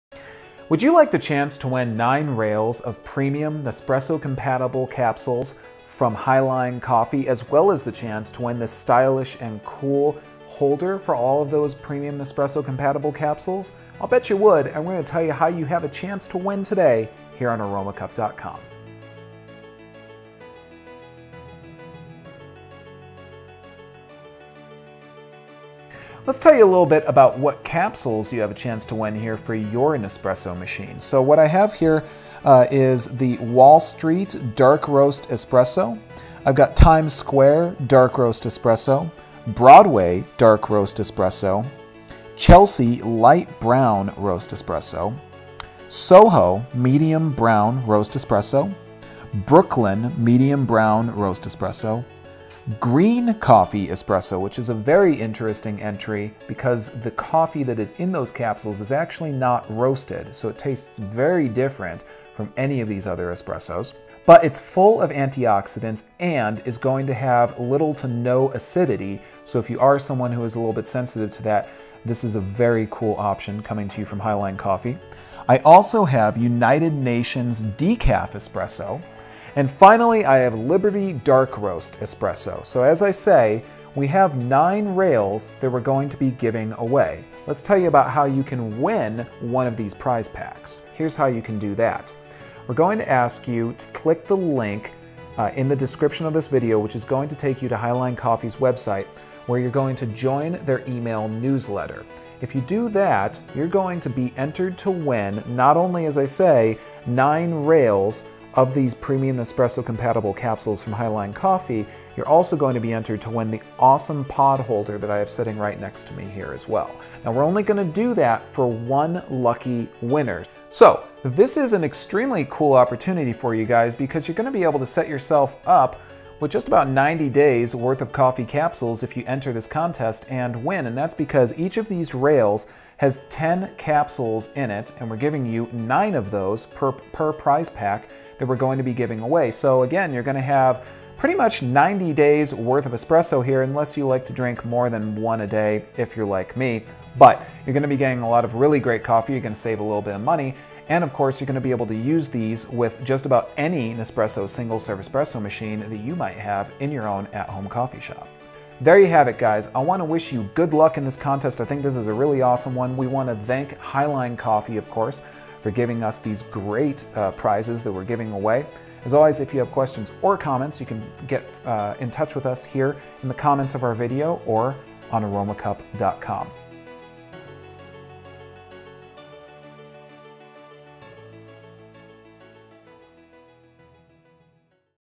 Nespresso Compatible Capsules GIVEAWAY by HiLine Coffee - Ends: 2/28/14
Enter for a chance to Win 9 Rails of Nespresso Compatible Capsules (see video for more details) plus Empire State Nespresso Capsule Holder by HiLine Coffee Company.

and signup for Newsletter. Once you signup you'll be automatically entered to Win 90 Nespresso Compatible Capsules plus Empire State Capsule Holder by HiLine Coffee Company. One signup one entry. No limits.

RULES: NO PURCHASE NECESSARY TO ENTER OR WIN. Purchase will not improve your chances of winning. MUST BE LEGAL U.S. RESIDENT AND 18 YEARS OF AGE OR OLDER TO ENTER. Sweepstakes ("Sweepstakes") is only open to legal residents of the United States and the District of Columbia. Void wherever prohibited or restricted by law.

PROMOTIONAL PERIOD: Sweepstakes begins at 12:00:00 noon (ET) on 1/31/14 and ends at 12:00:00 noon (ET) on 2/28/14. Winner will be determined by a random drawing conducted by a Aromacup.com representative on 2/28/14.  Winner will be announced within 5 business days and will be notified by e-mail. Each winner will have five (5) days from notification to accept the prize by e-mail. Aromacup.com shall not be liable for unsuccessful efforts to notify a winner.